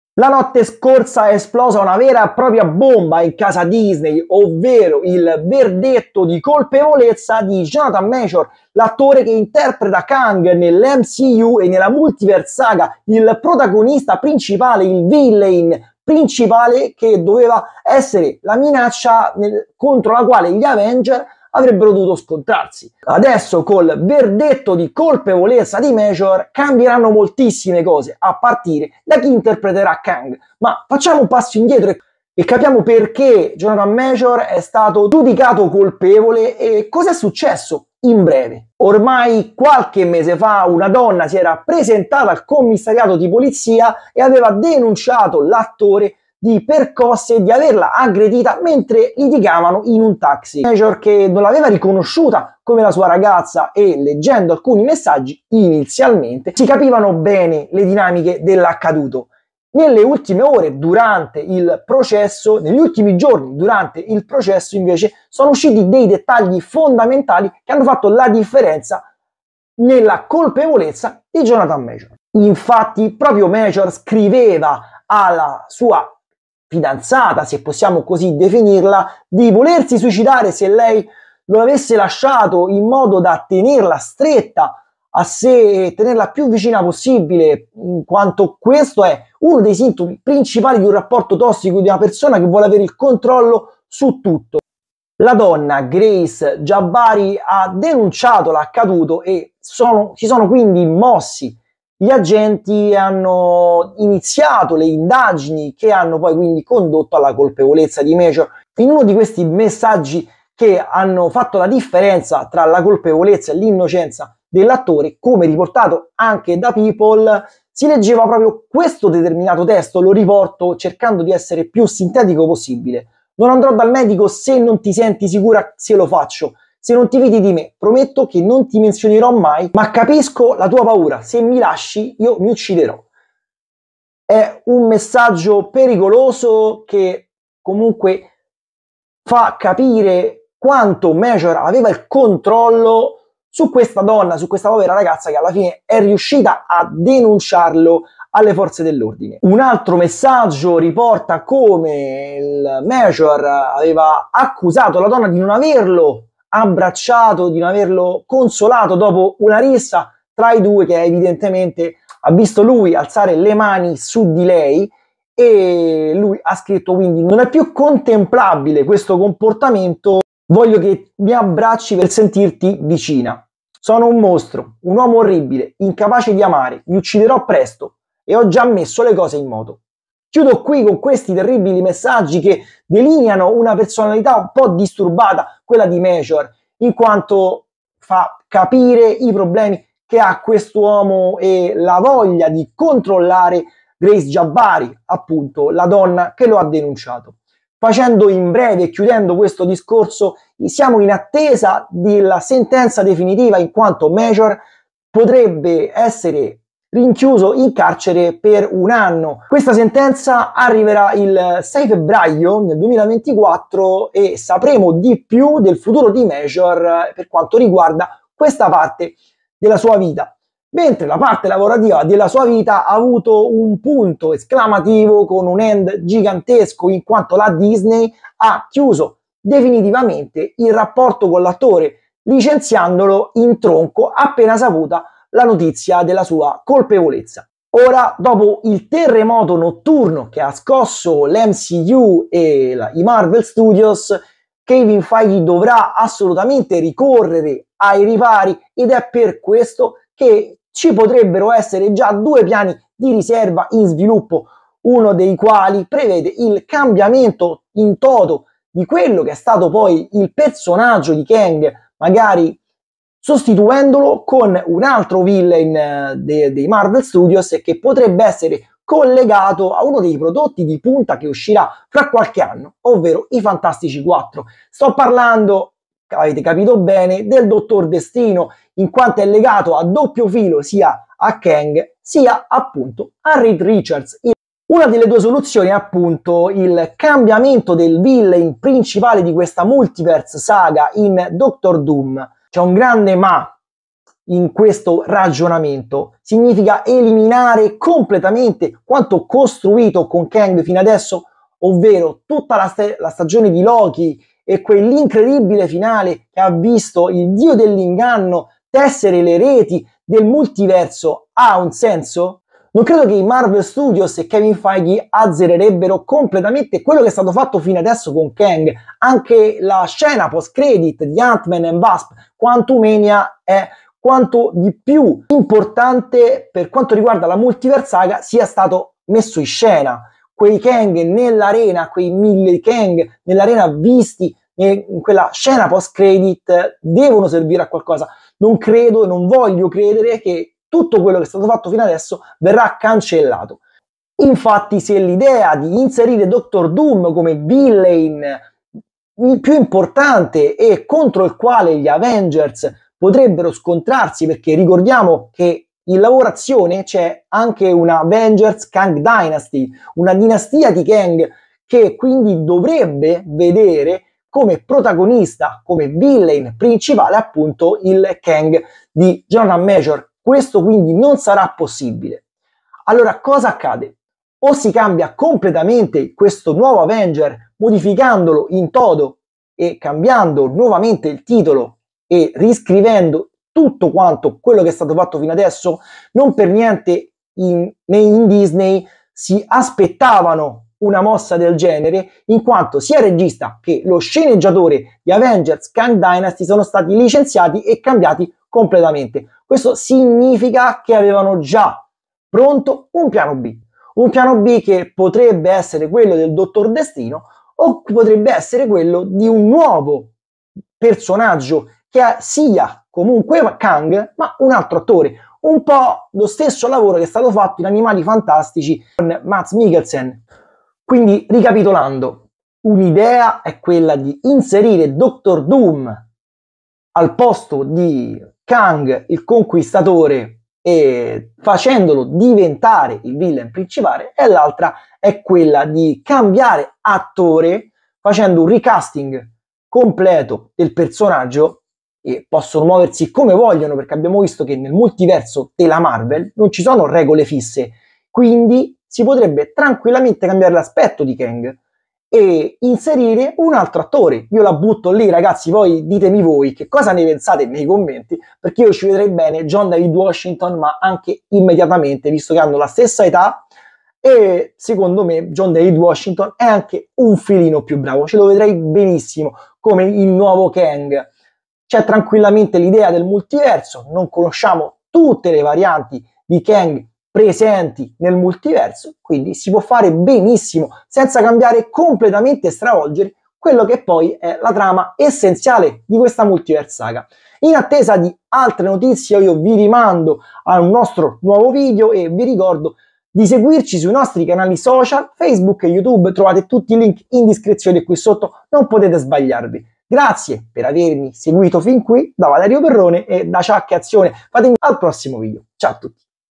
Marvel, Jonathan Majors CONDANNATO e LICENZIATO! Ecco perché
Marvel ha licenziato Jonathan Majors dopo la condanna per aggressione e molestie alla fidanzata. Ci racconta tutto il nostro Valerio Perrone!